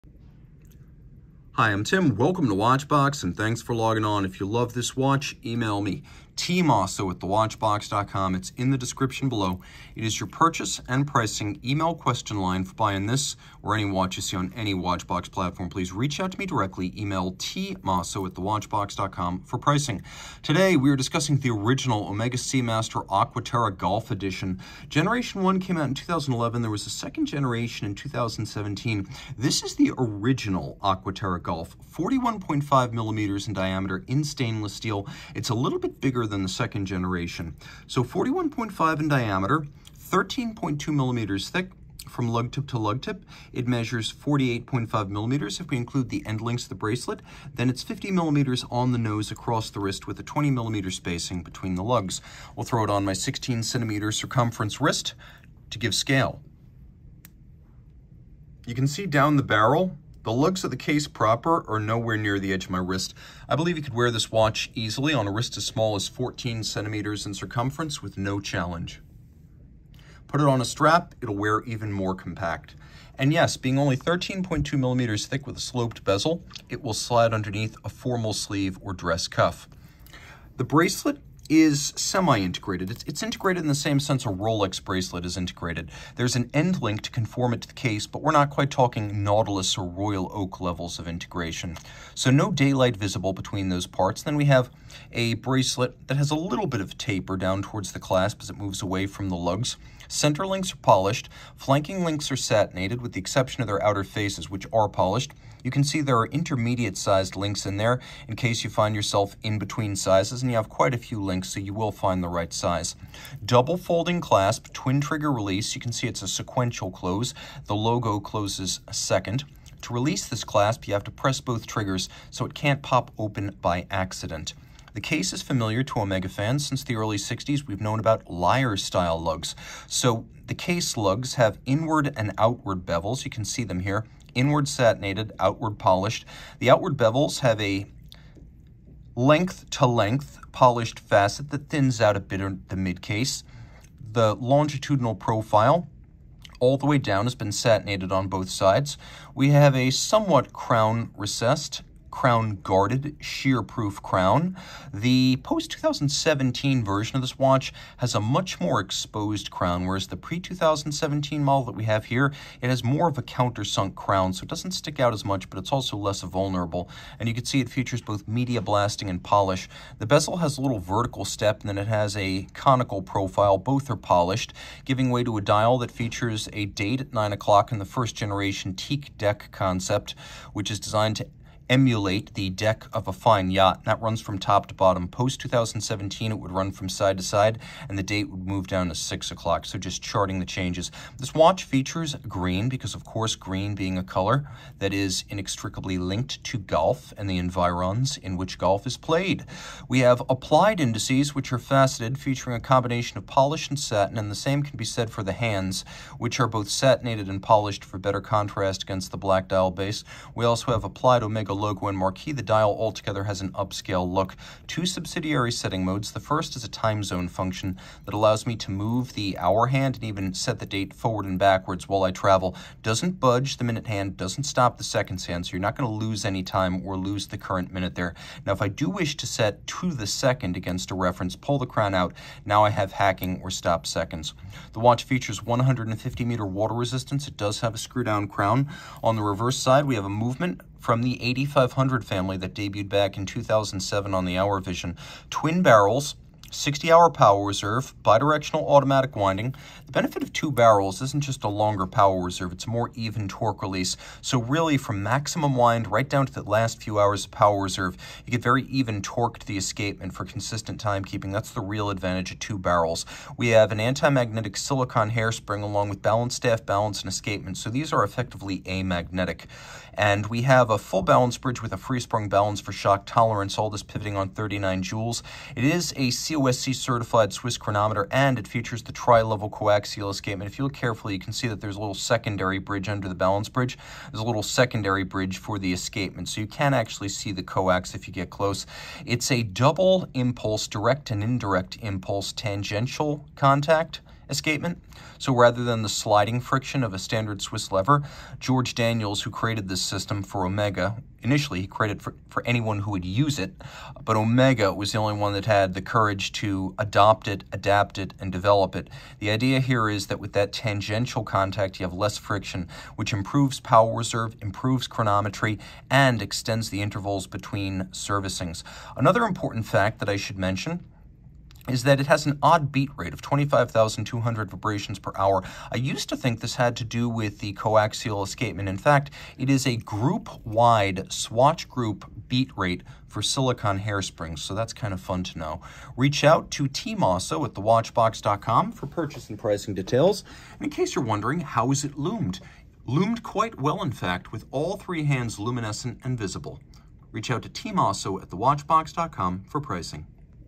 Omega Seamaster Aqua Terra 150M "Golf Edition" 231.10.42.21.01.004 Omega Watch Review
SKU: 4791836

The Omega Seamaster Aqua Terra 150M "Golf Edition" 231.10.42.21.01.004 is encased in 41.5mm of stainless steel surrounding a black dial on a stainless steel bracelet. Features of this Omega Seamaster Aqua Terra 150M "Golf Edition" include hours, minutes, seconds, and date.

For complete details, watch the full Omega Seamaster Aqua Terra 150M "Golf Edition" review.